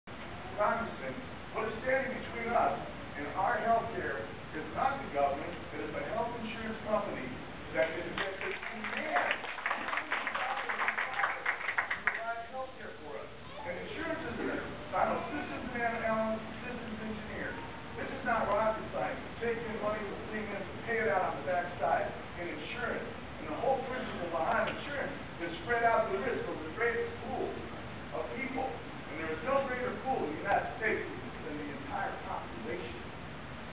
Part 2: What is standing between us and our health care...
"Congressman, what is standing between us and our health care is not the government; it is the health insurance companies. " (applause)
Jim is speaking at Jack Kingston's (R GA-01) Health Care Town Hall in Valdosta in August 2009.
Video by Gretchen Quarterman.